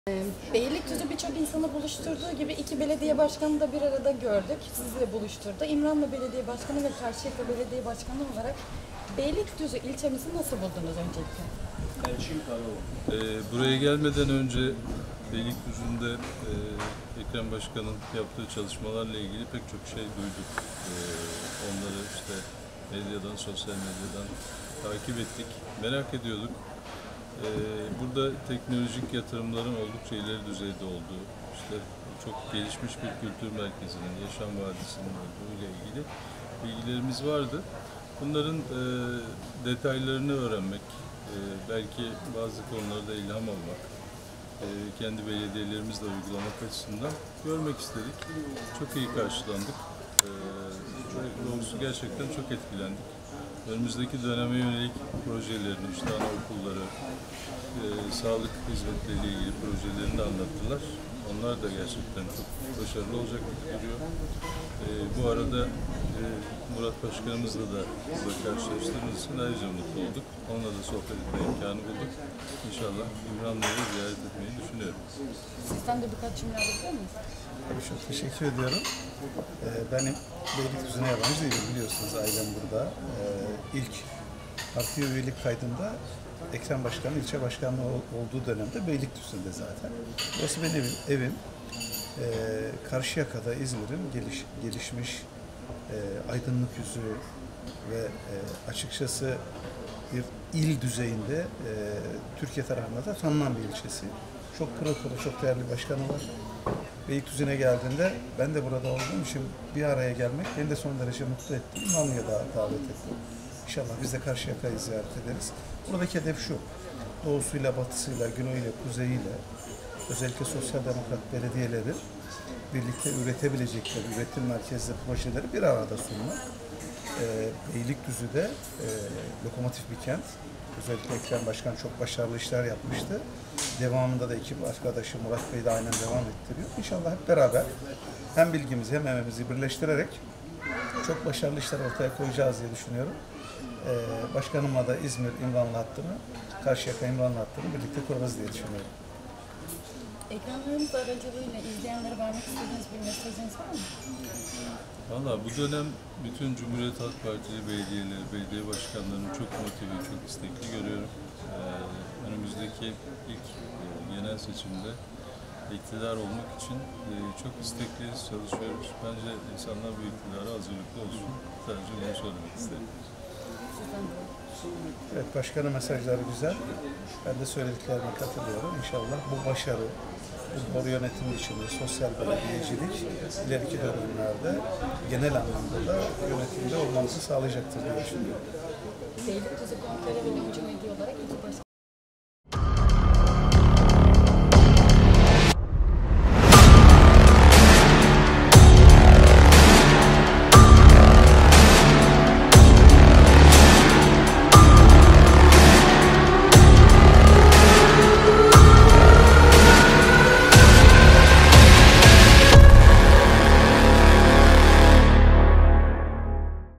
Murat Açıl ve Dr. Cemil Tugay röportaj
İzmir Karşıyaka Belediye Başkanı Dr. Cemil Tugay ve Sivas İmranlı Belediye Başkanı Murat Açıl Beylikdüzü ilçesinde incelemelerde bulundular. Lonca Medya mikrofonlarımıza değerlendirmede bulundular